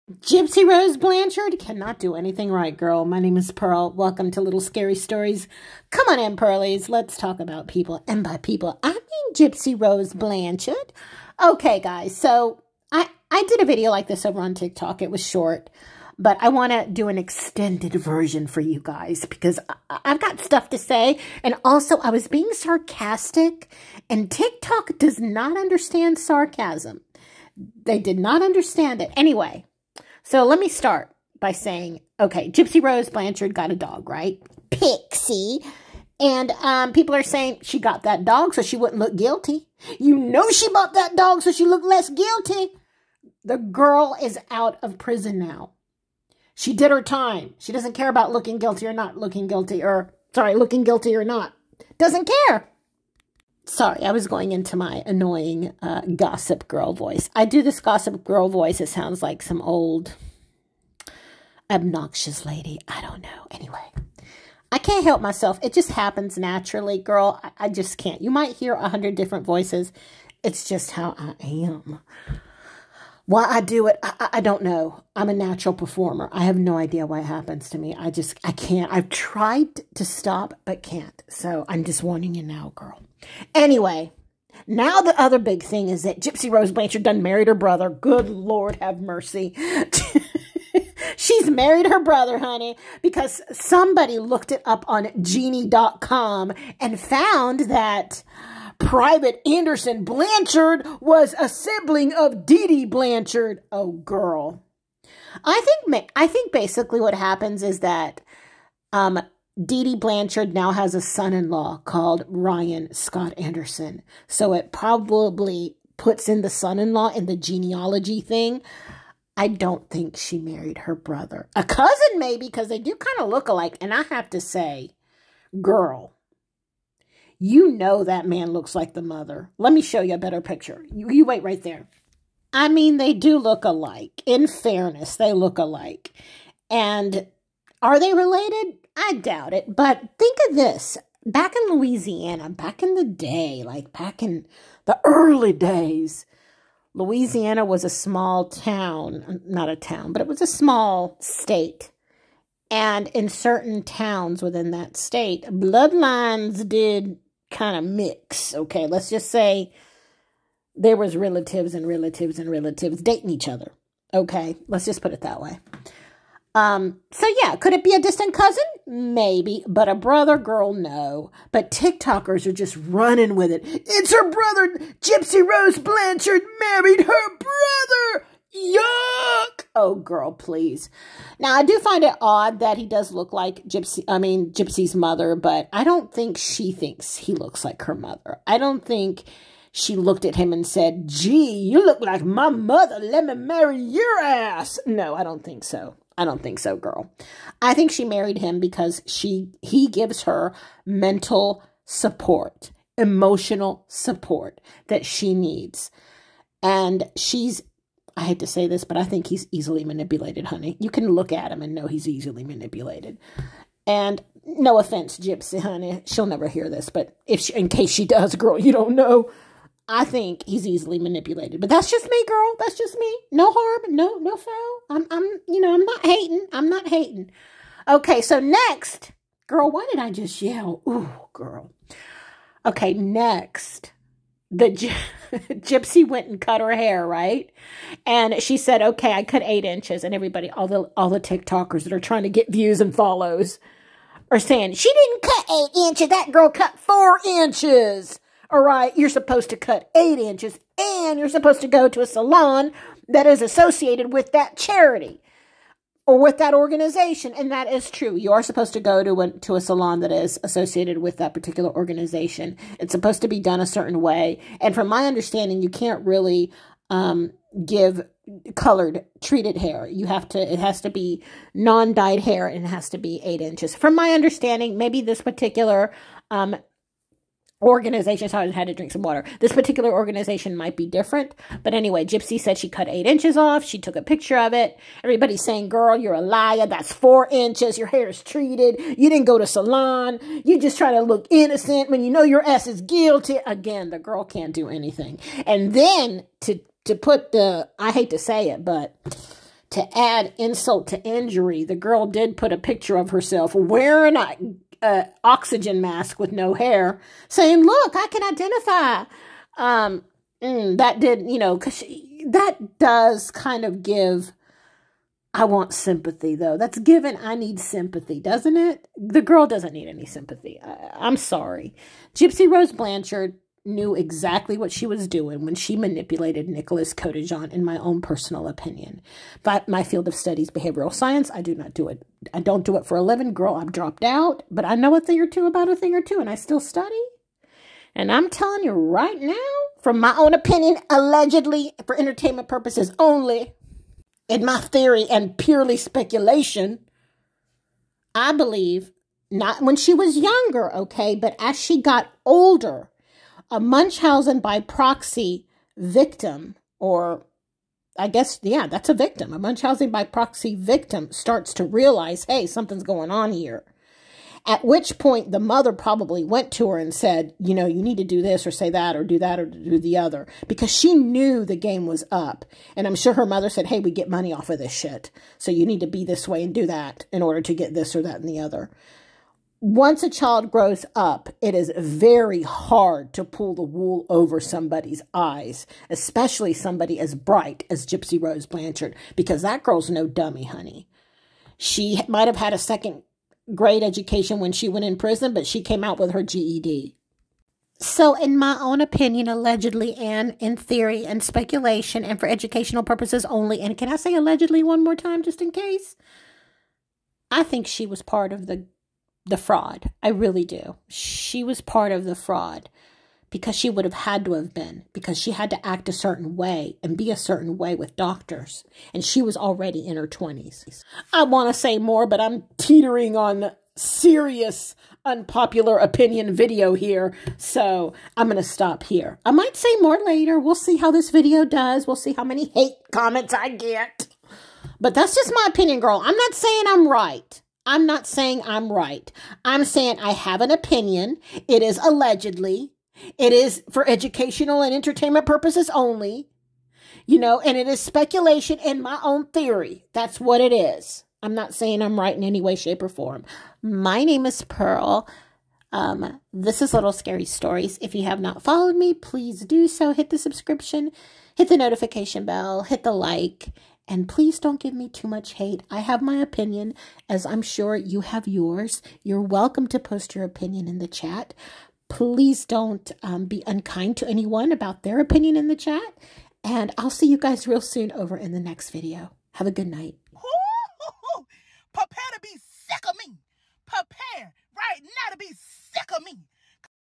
Crazy Gypsy Rose Blanchard Rumors and my unpopular opinion.
Creators are hating on every post Gypsy makes. Let's discuss. Also my unpopular opinion on her case. Everything in this video is
Allegedly.